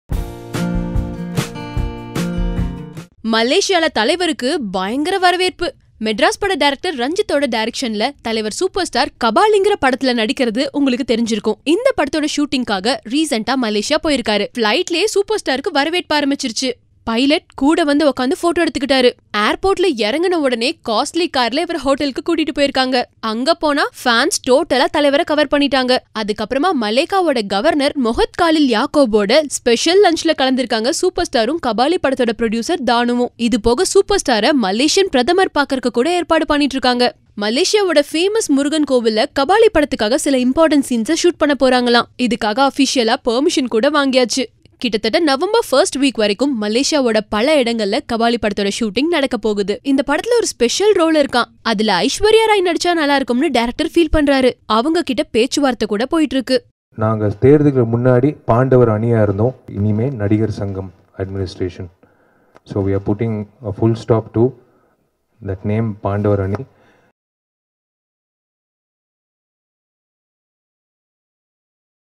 Malacca Governor hosts lunch for Rajinikanth | Kabali Malaysia Shooting
Rajinikanth left for Malaysia yesterday to shoot for his next film 'Kabali' which is being directed by Ranjith of Madras fame. Social media has been buzzing with pics of Rajinikanth in Malaysia and welcome messages. He was welcomed by the governor of Melaka, Mohd Khalil bin Yaakob. Interestingly, the governor also hosted lunch for Rajinikanth.